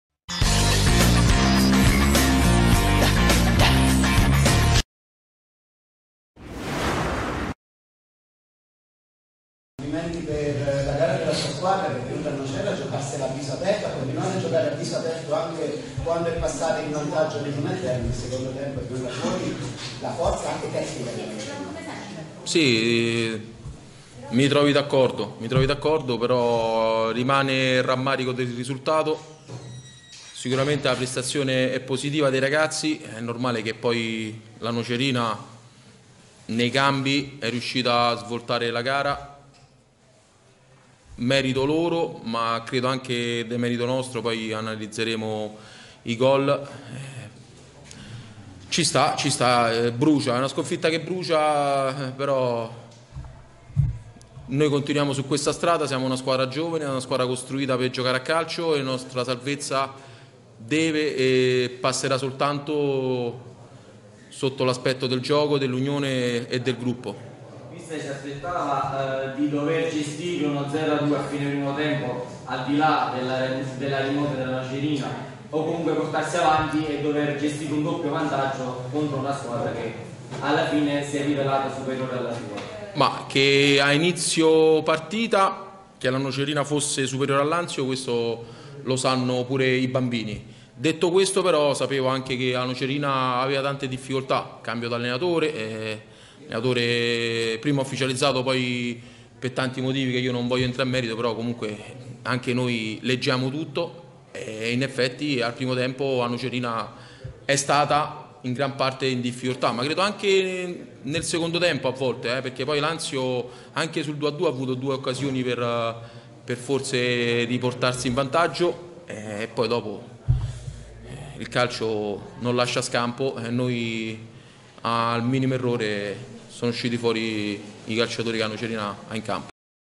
CONFERENZA STAMPA: Nocerina - Anzio 4-2 Mr. Mario Guida
Al termine della gara tra Nocerina e Anzio finita per 4-2 per i molossi, è intervenuto ai nostri microfoni il mister dell'Anzio Mario Guida, ecco le sue parole